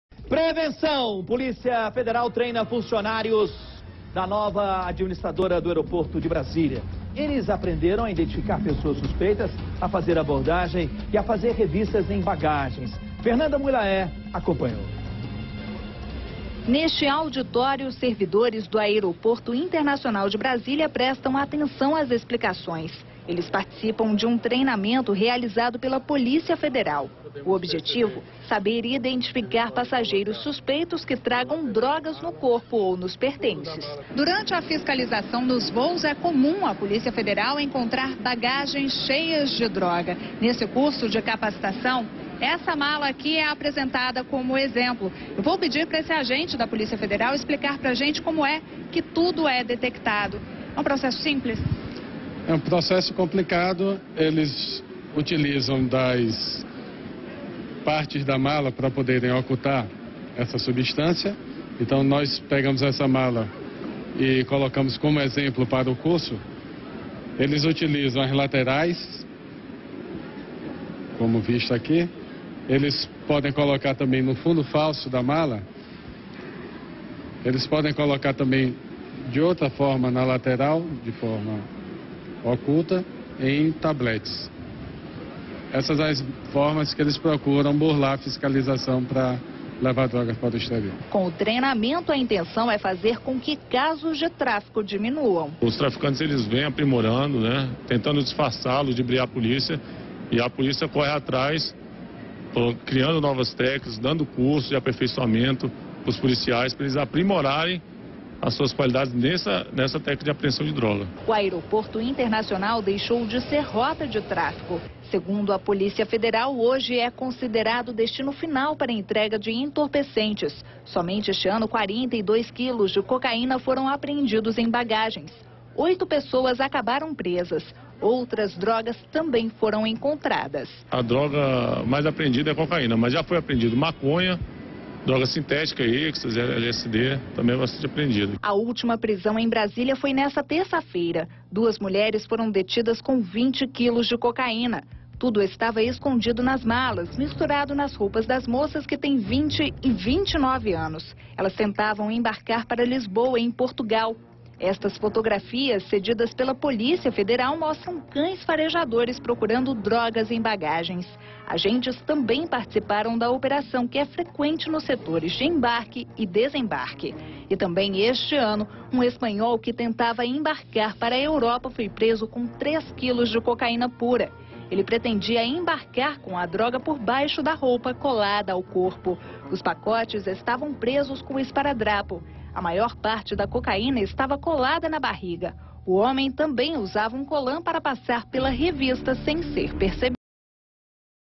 Servidores do Aeroporto passam por treinamento com a Polícia Federal
Com o treinamento, eles aprenderam a identificar pessoas suspeitas, fazer abordagens e revistas em bagagens.